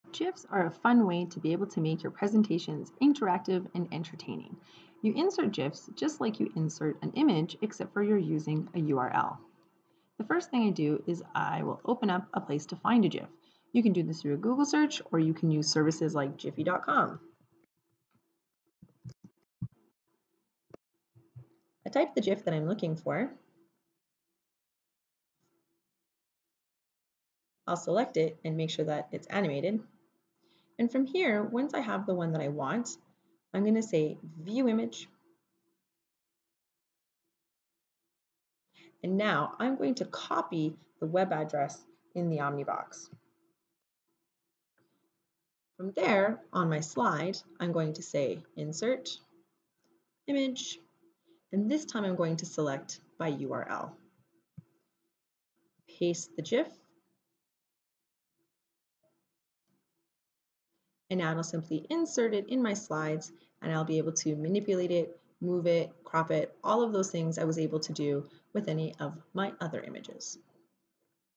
Inserting Gifs in Google Slides
In this Getting Started with Google Slides video you will learn how to add gifs! For more help getting started with all the Google for Education tools please